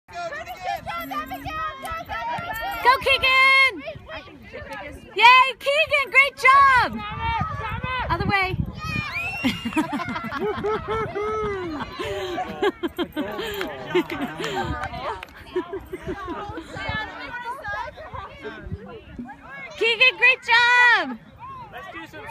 Keegan got a goal in soccer!
25 April 2015 -
Keegan gets his first game time goal in his life. Its in the wrong goal- but who cares. He promised me before going in this time that he would get a goal for me, and he did.